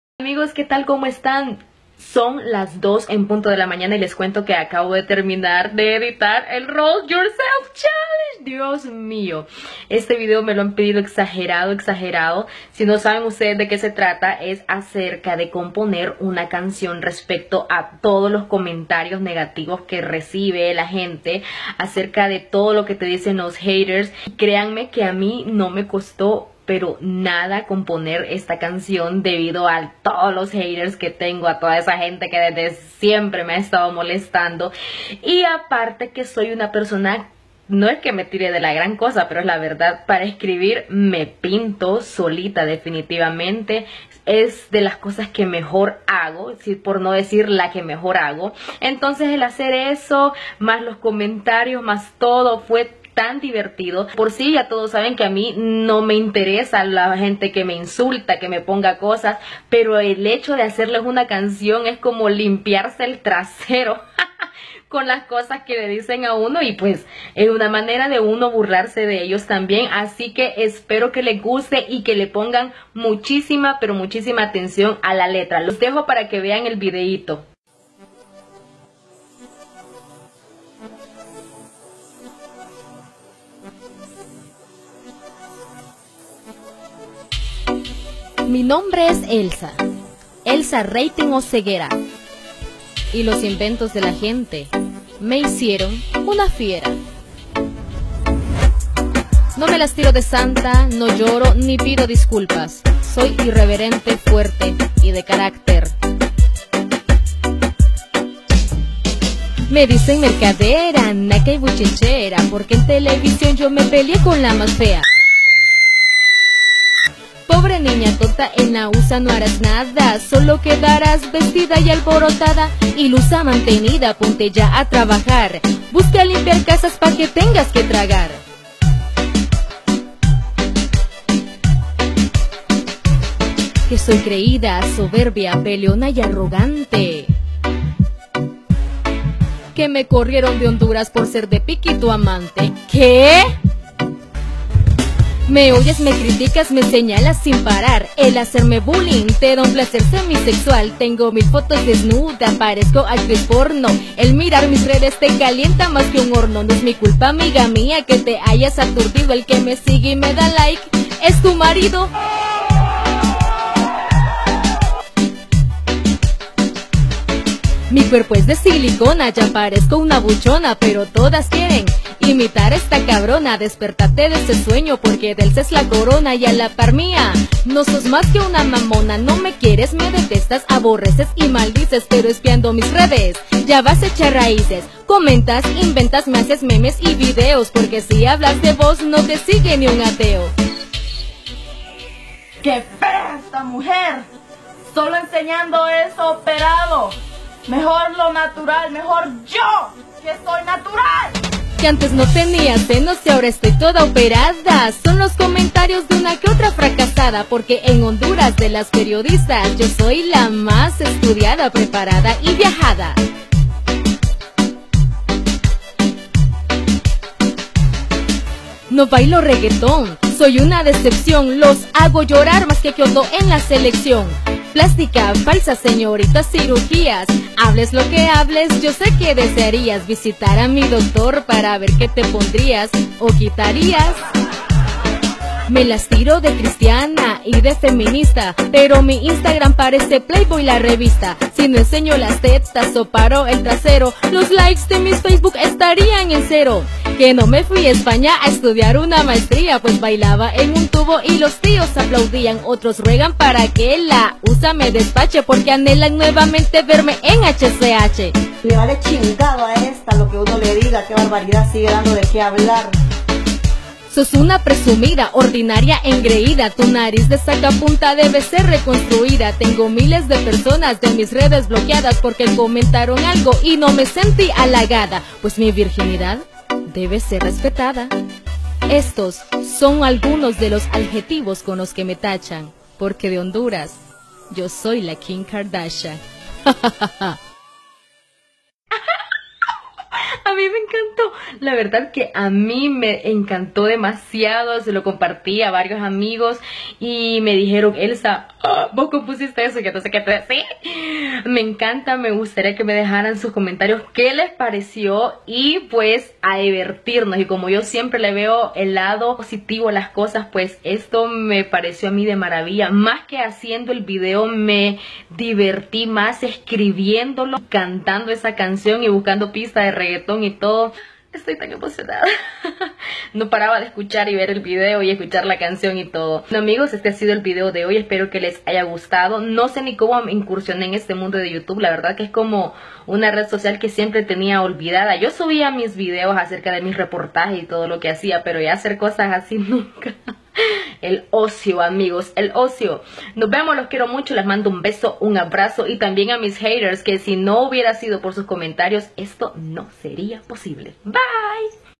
Elsa Oseguera ataca a periodista y dice ser la más estudiada, preparada y viajada
La controversial y hermosa periodista Elsa Oseguera estrenó un polémico video dedicado a todos sus “haters”, incluyendo a una excompañera de trabajo y al periodista que según ella publicó varias fotografías suyas desnuda.

Mas noticias en Diario El Articulista